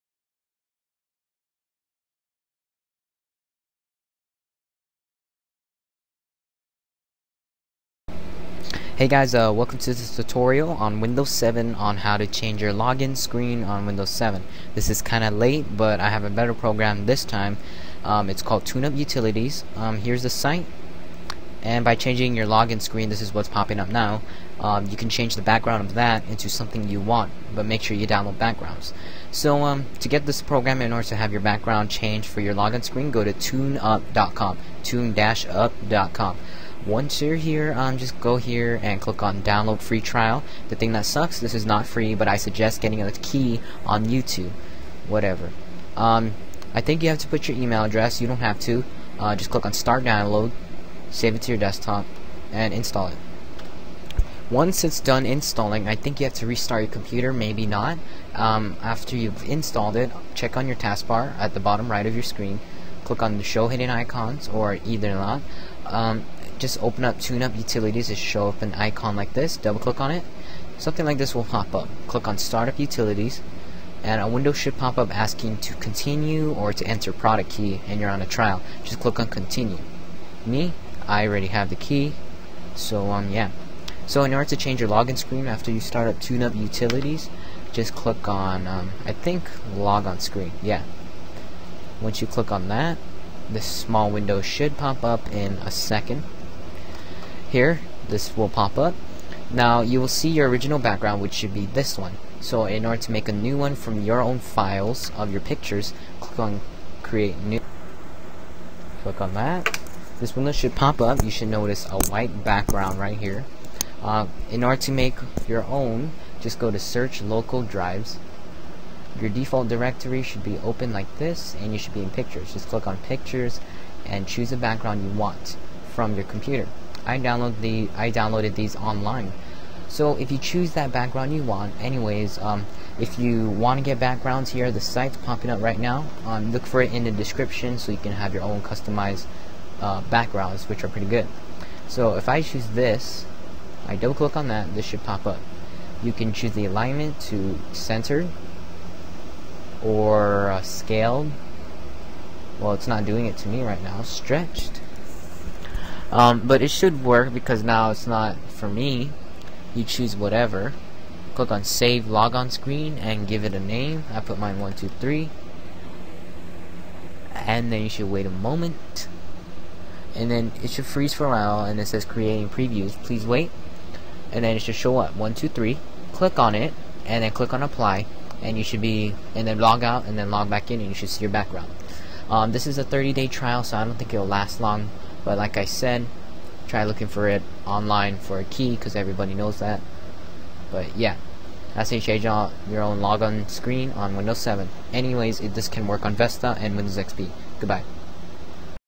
How to change your Login Screen on Windows 7/ Vista / XP
In this video I will show you how to change your login screen on

Windows 7
Windows Vista
Windows XP

Program
TuneUp Utitlites

It's a 30 day trial

So what a bummer...